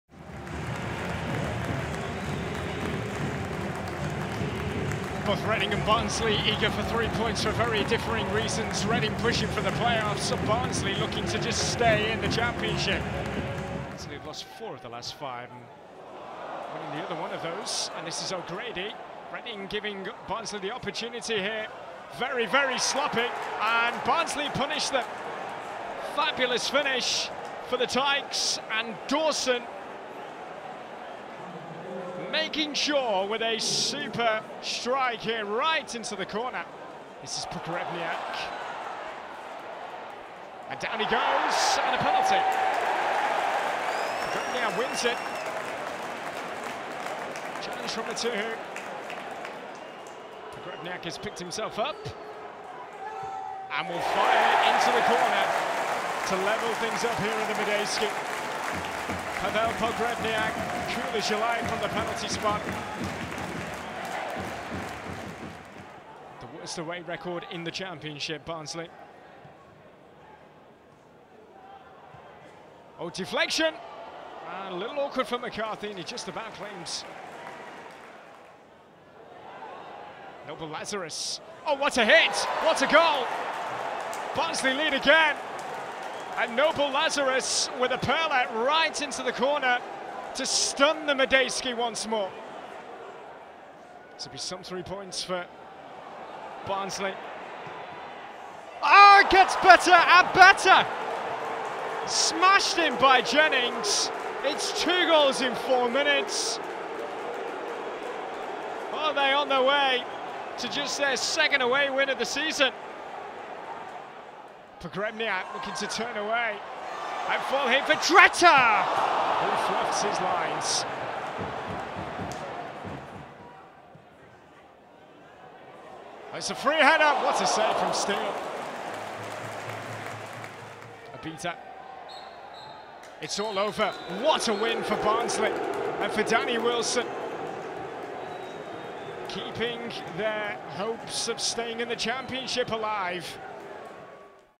Reading vs Barnsley - Championship 2013/14
Reading 1-3 Barnsley.

Highlights from Reading's Sky Bet Championship match against Barnsley at the Madejski Stadium.